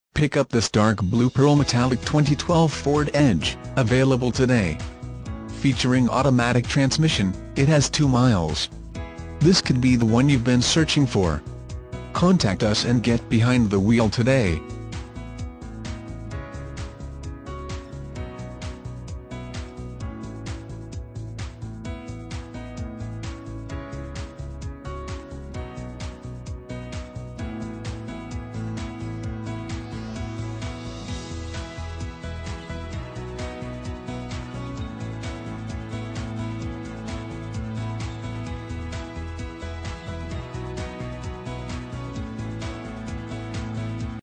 2012 FORD EDGE Saint Albans, WV FT2151
2012 FORD EDGE Saint Albans, WV
Stock

Moses Ford
2001 MacCorkle Ave SW

Pick up this Dark Blue Pearl Metallic 2012 Ford Edge, available today. Featuring automatic transmission, it has 2 miles. This could be the one you've been searching for.